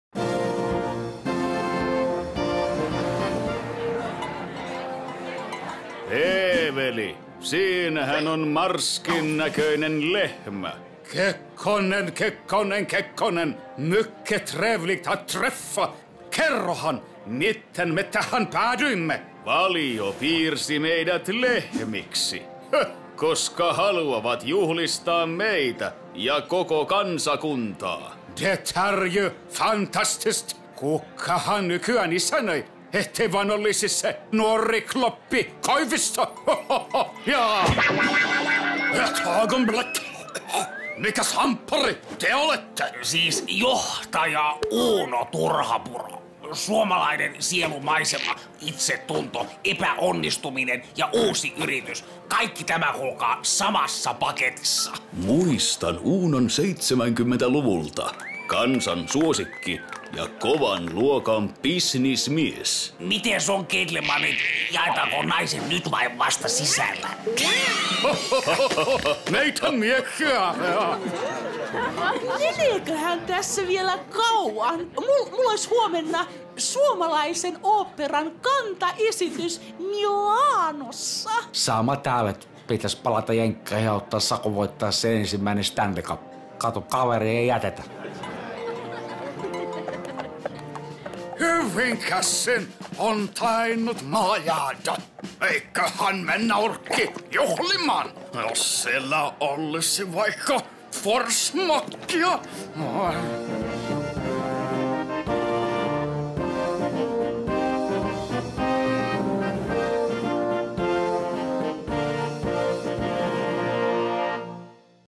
Valio / Suuret Suomalaiset / Erikoislähetys
2 minutes of animation in 2 weeks. Remember everyone, that's not the usual schedule. Anyhow, all in all this was a great project and our thanks go to the teams at SEK & Valio & MTV3 for working so closely with us throughout the creative process for these independence day cow specials!

Animation production: Anima Boutique
Additional design: Joonas Utti, Eliza Jäppinen
Lead animator: Joonas Utti
Animation: Kiira Kalliomäki, Mari Tolkkinen, Heli Ellis
Backgrounds: Antero Nuutinen
Edit: Anttu Harlin, Mari Tolkkinen
Comp: Heli Ellis, Mari Tolkkinen
Producer: Anttu Harlin

Client: Valio
Agency: SEK & Grey
Script: SEK & Grey
Voice talent: Jarkko Tamminen
Sound: Timo Anttila / Humina
Character design: Ville Granroth